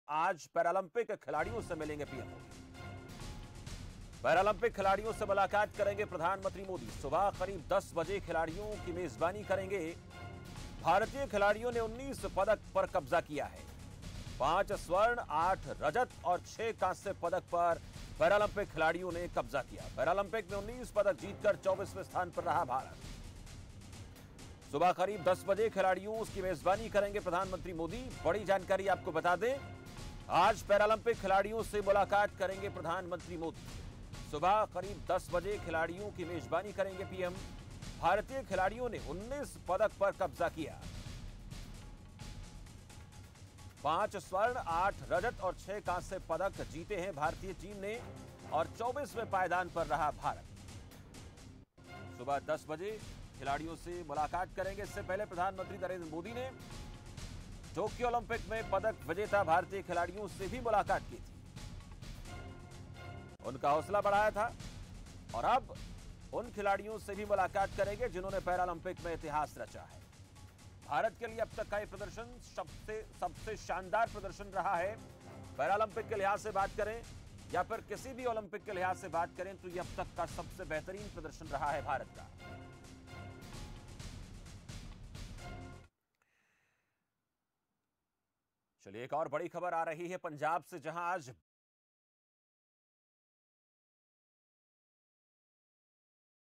भारतीय paralympic में खिलाड़ियों का ऐतिहासिक प्रदर्शन, 19 medals पर किया कब्जा
भारतीय paralympic में खिलाड़ियों का ऐतिहासिक प्रदर्शन
19 medals पर किया कब्जा
आज खिलाड़ियों से मिलेंगे PM मोदी